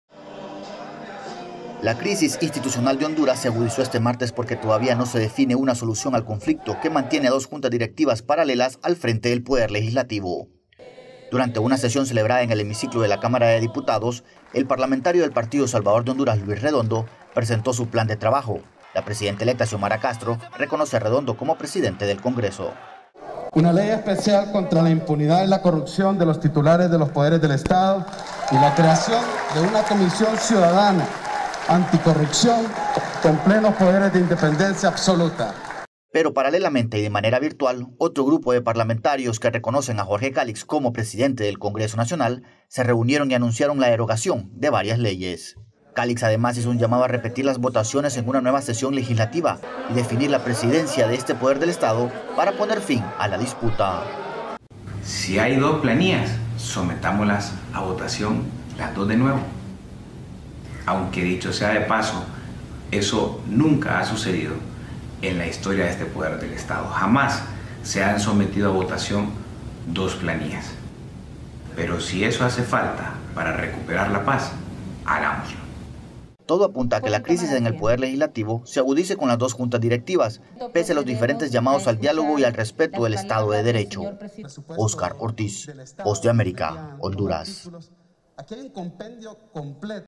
Doble presidencia del Congreso en Honduras
La crisis política de Honduras se profundizó el pasado martes, cuando las dos juntas directivas paralelas del Congreso Nacional sesionaron de manera simultánea pero separadas, sin poner de lado sus diferencias, dos días antes de que Xiomara Castro asuma el poder.

¡Visítenos!